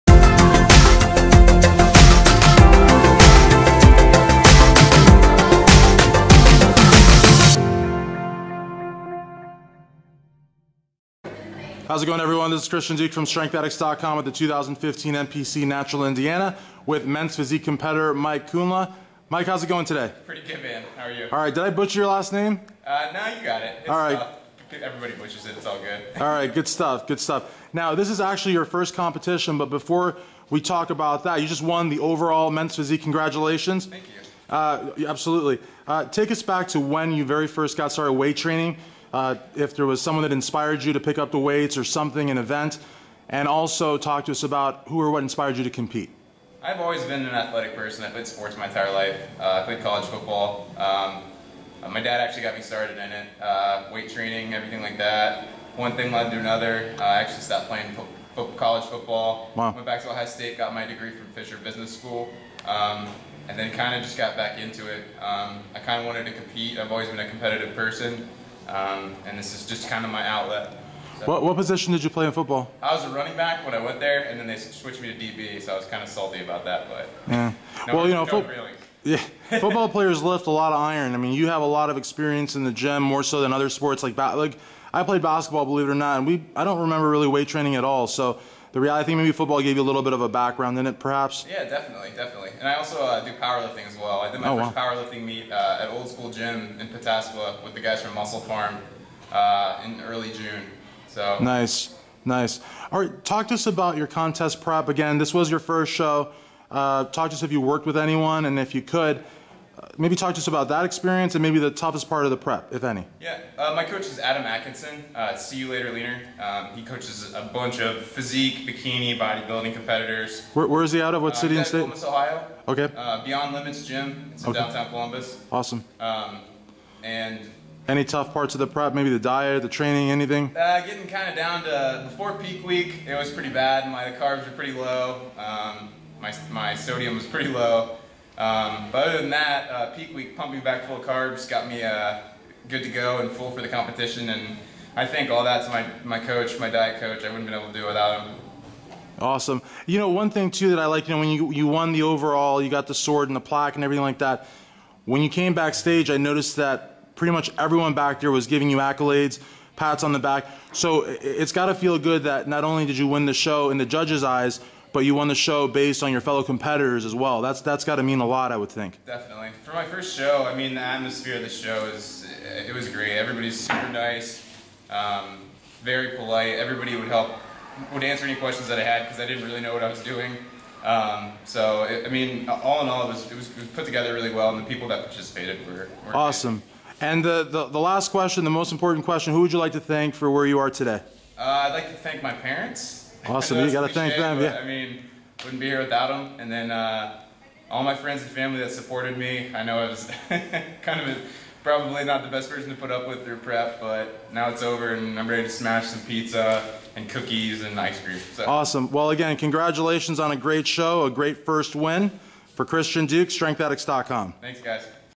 Mike Kundla - 2015 NPC Natural Indianapolis (Interview)
This was MIKE KUNDLA's first show and he won his class & the overall. Yes, that's right - first show - WON IT ALL!! Check out this great interview immediately after Mike got off stage.